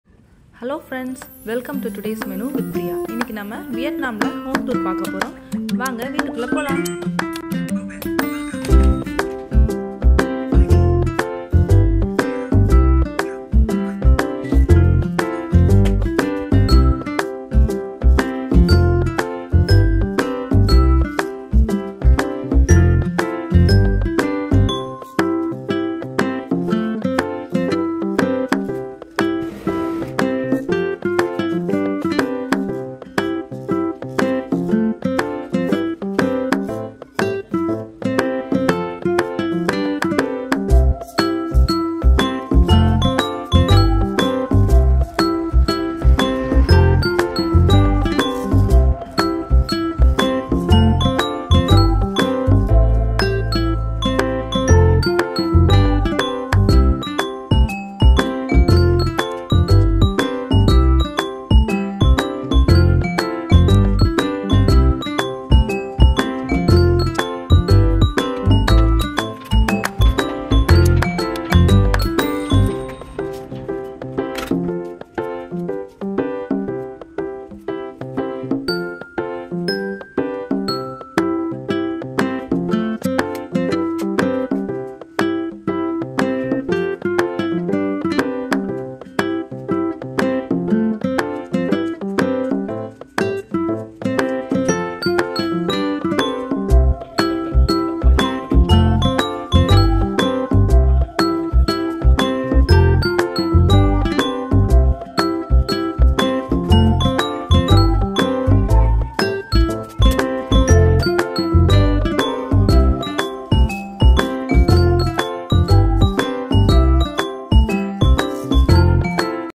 Home tour in vietnam
In this video we will be seeing a home tour in vietnam.